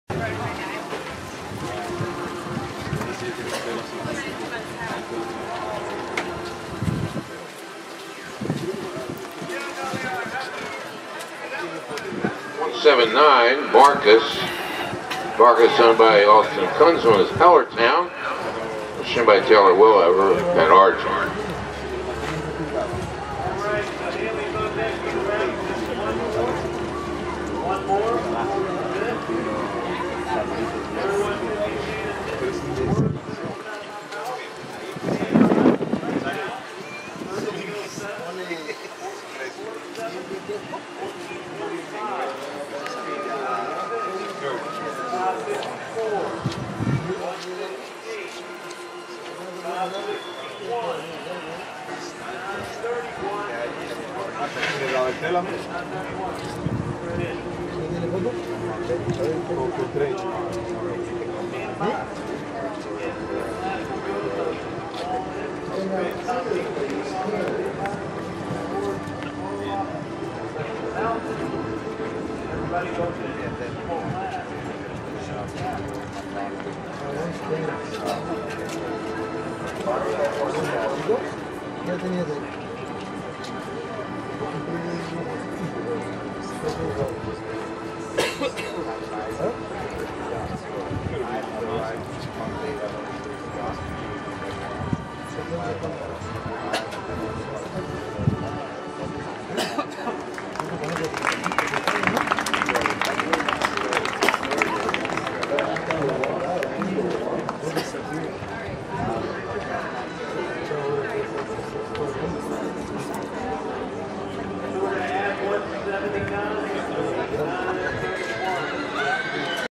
Video of MARKUS ridden by TAYLOR WILLEVER from ShowNet!
MARKUS ridden by TAYLOR WILLEVER at Devon Sr Week 2017.  This round is from class 184, $250 LOCAL HUNTER STAKE SECTION A and finished 1 of 30.